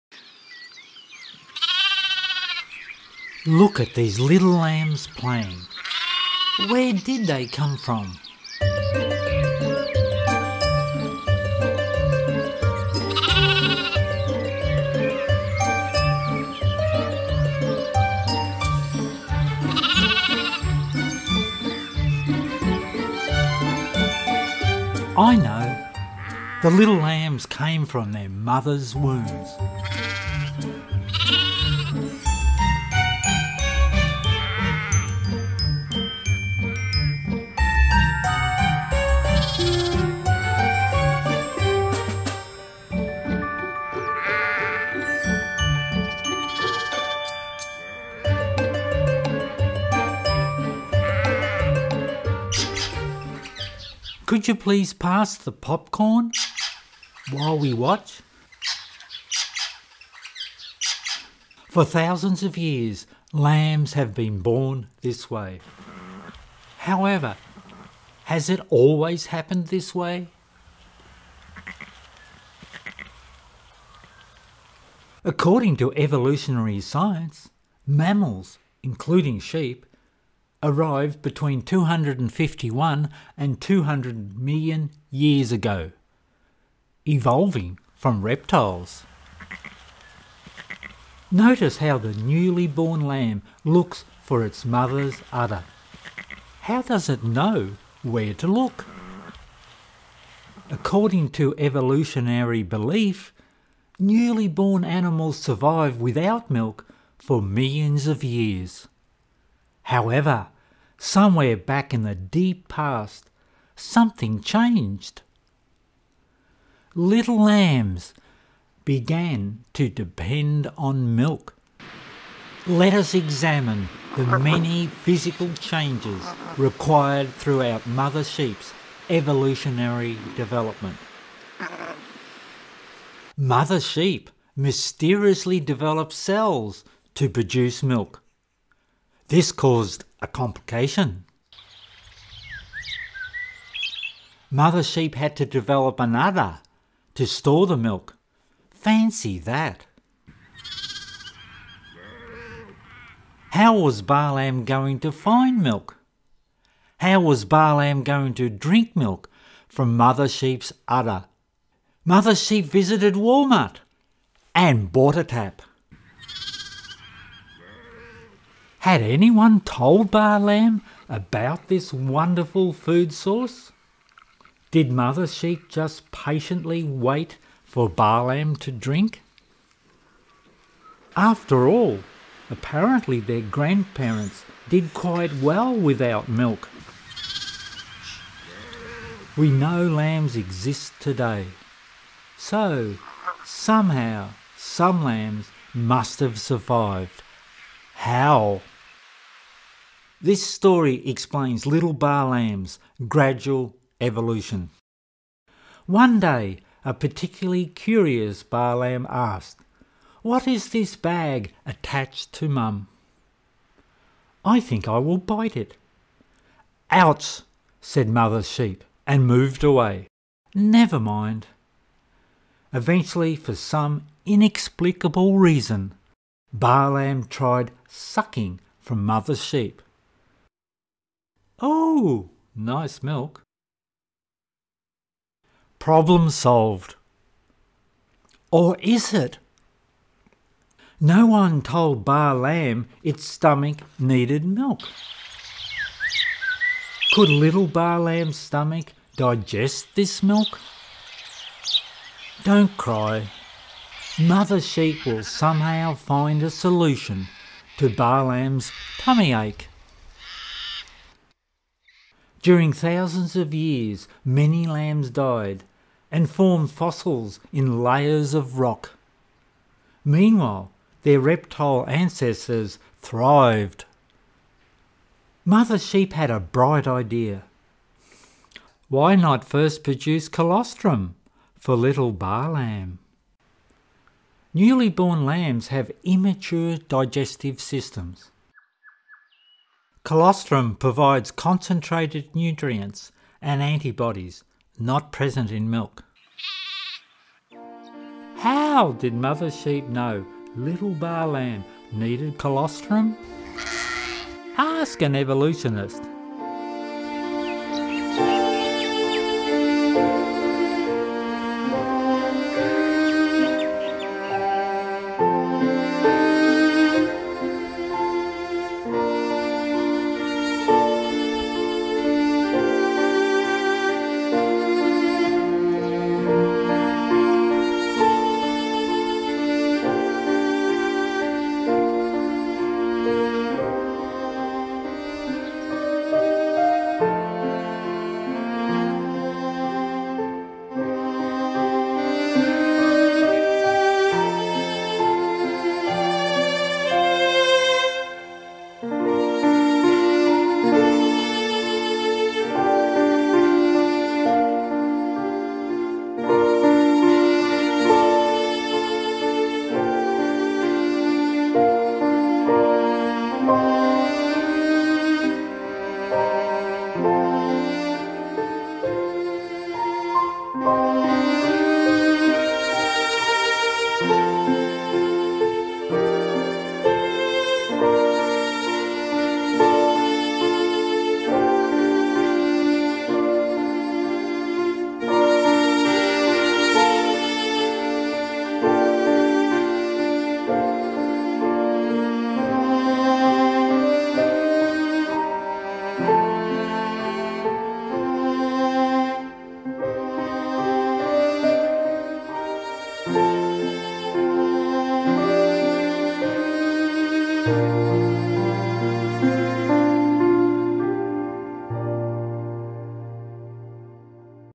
Lamb Evolution
Cute little baby lambs play. How did these happy lambs originate? Sheep characteristics. Lambs sleep in peace. How did sheep begin to produce milk? How could lambs have evolved to drink milk from sheep?

Mother Sheep gives birth to twin babies. Colostrum is critical to lambs' survival. Why do ewes produce milk and why do lambs drink milk?

Is the theory of evolution logical? Does God's beautiful creation of life offer a better explanation?

Christian free online education
Common Faith Academy